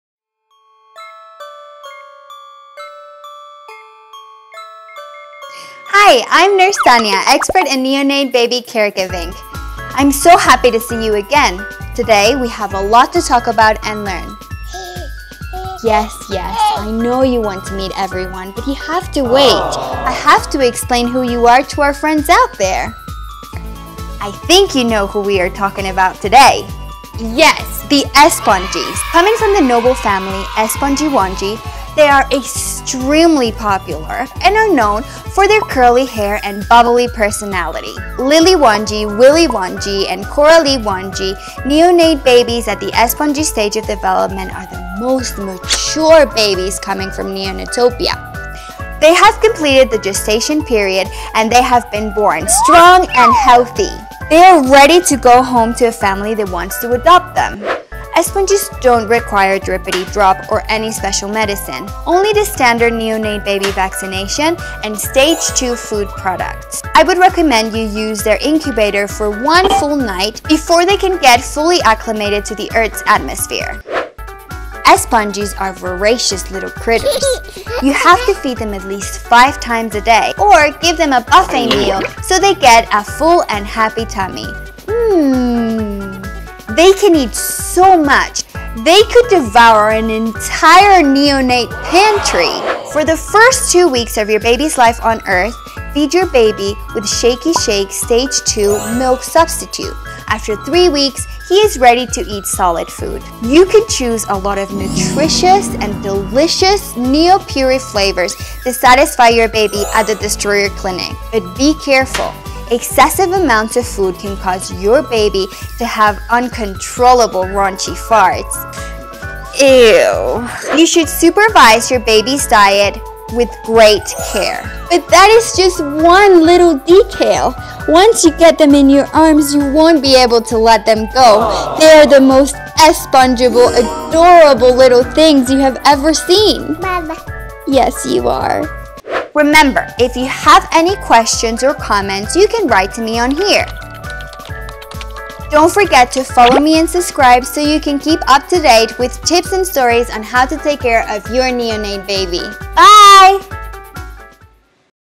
How to Take Care of Your Espongie Baby
Nurse Tania explains how to care for your Espongie baby. Neonate Babies at the Espongie stage of development are the most mature of all the babies from Neonatopia. They are the most cuddliest of the bunch!